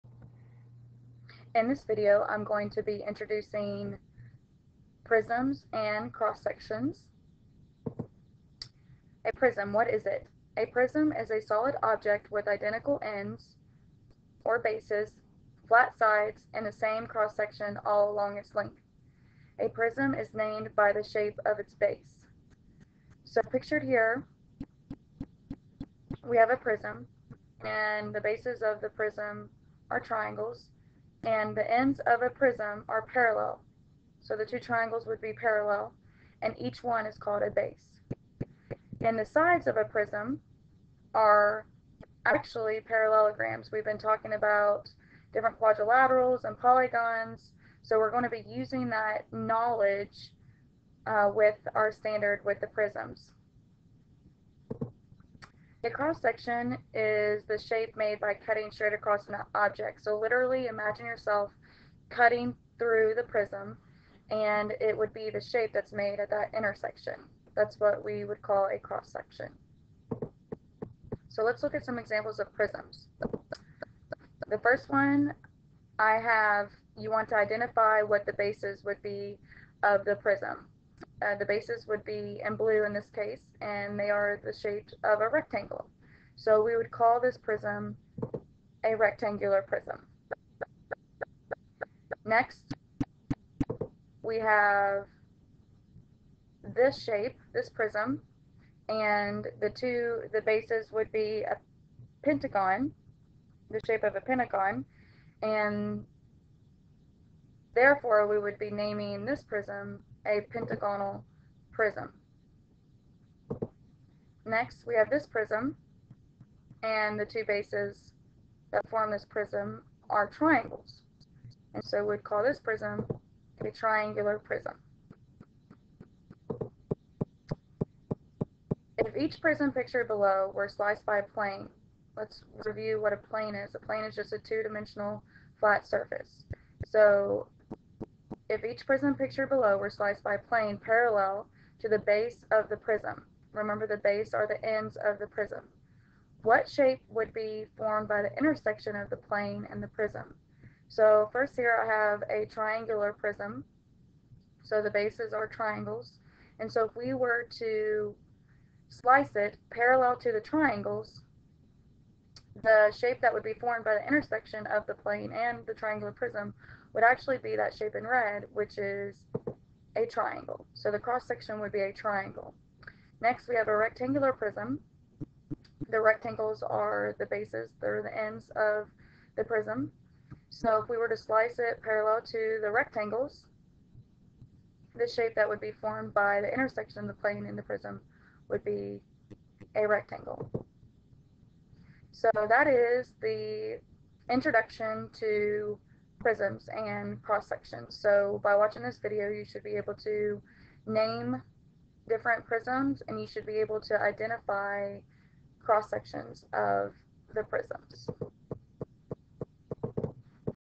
Prisms and Cross Sections
Watch this video to learn how to name prisms and identify cross sections.